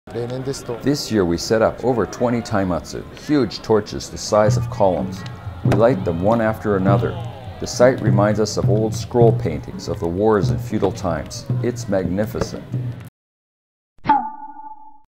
025E -The Taimatsu Akashi No.2 [Ganbatte 365: Follow Japan renewal after Earthquake and Tsunami]
The Taimatsu Akashi fire festival has been held in Sukagawa every year for over 420 years. This year it looked like it might not be possible to hold the festival, but help from all around the country made it possible. Originally a memorial service for warriors from the Samurai era, it has turned into a dramatic event that reminds us a bit of what it must have been like in those feudal days. This year the festival has a special purpose, to remember victims of the disaster. Also, as a sign of recovery, it unites the people of Fukushima.